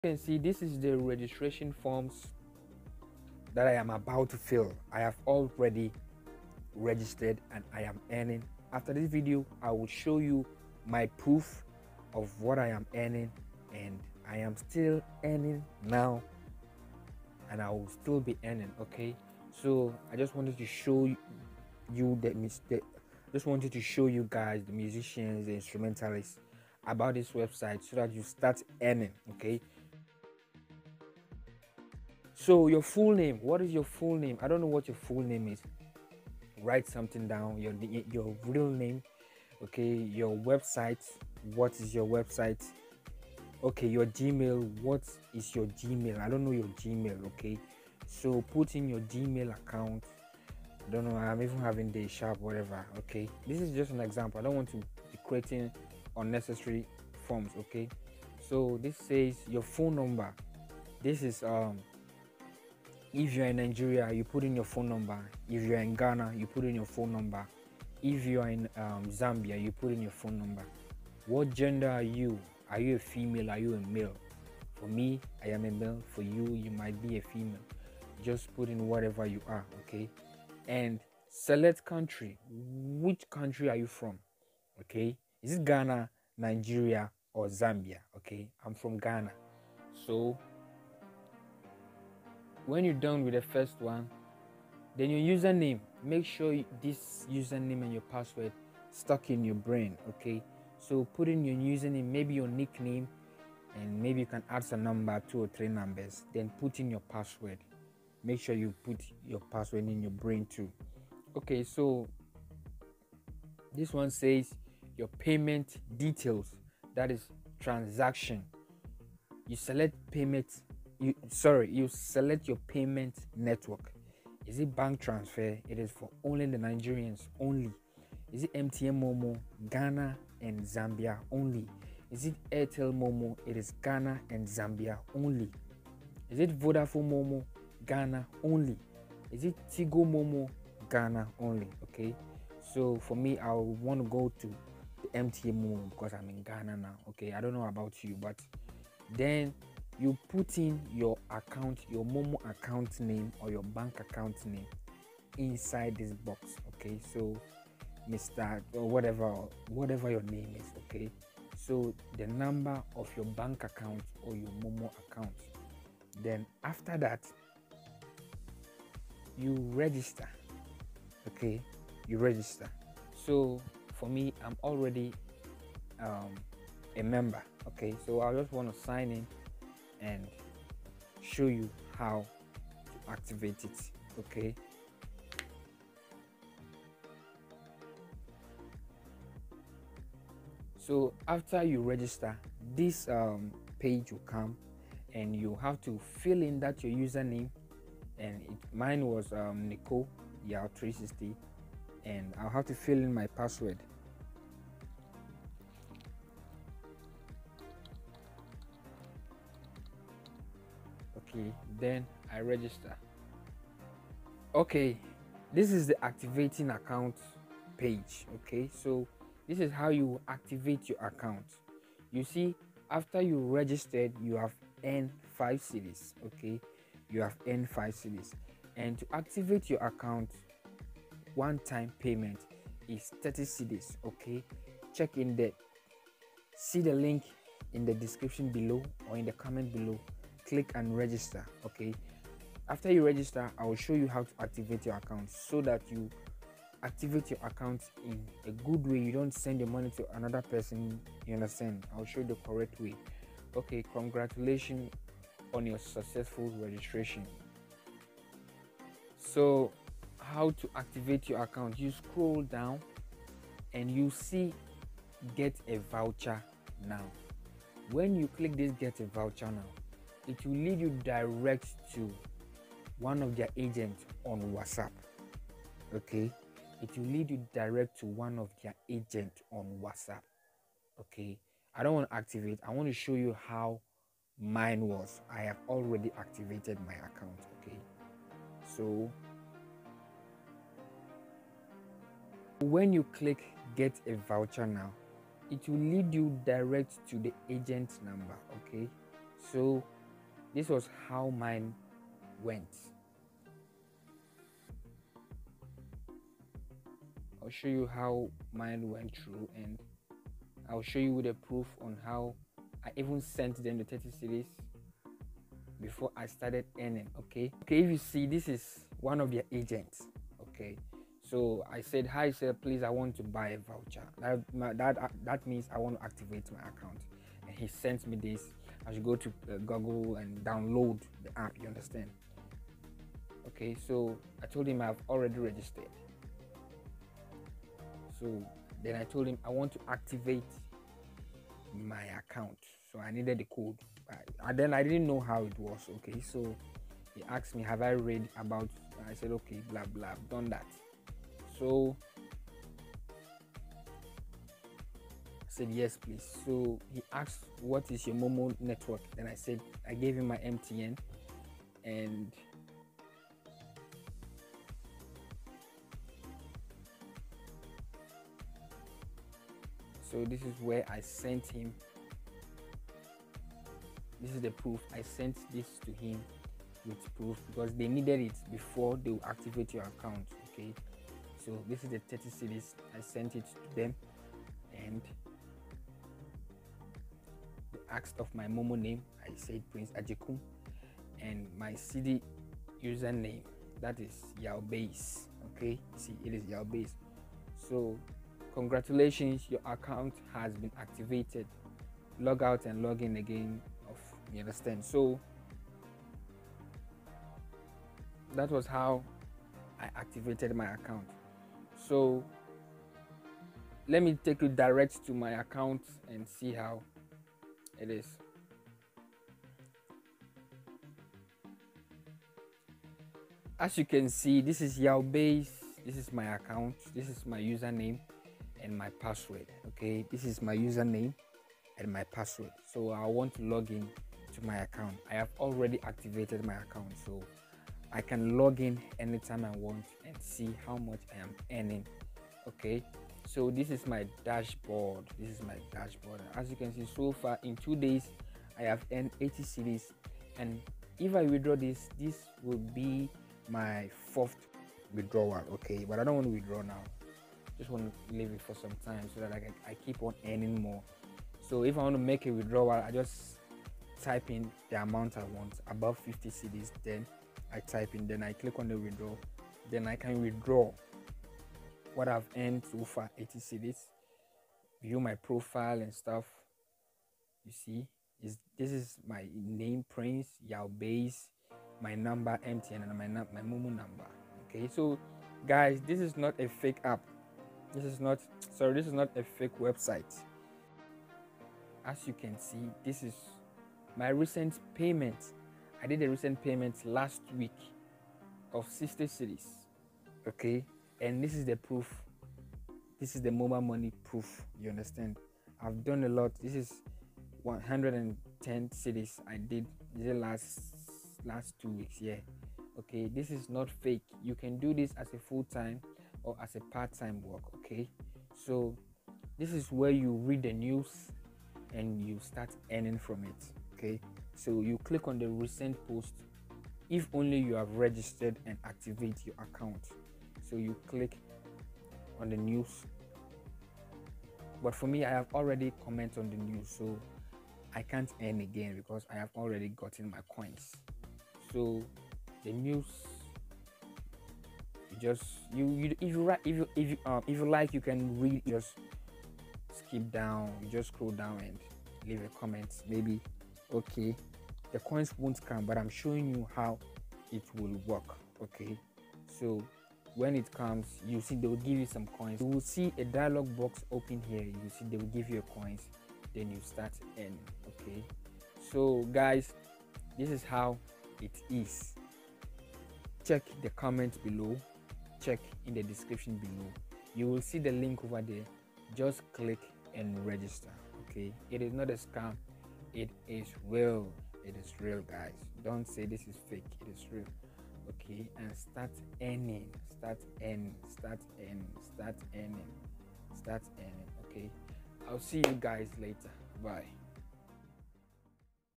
Earn CEdis Into Your Momo Account While you Wait For Gigs...Musicians(Instrumentalist) 💯💯% Legit🔥🔥💸💸
Register & Activate Your Account. 💸💸💸💸💸💸

●In This Videoi have shared a side hustle opportunity we're you can earn money as a MUSICIANS as You Read and comment on news as you wait for a Gig. A Part time Online work From Home.

BIG SORRY FOR THE DISTRACTION(COUGHING) I WAS NOT FEELING WELL AT THAT MOMMENT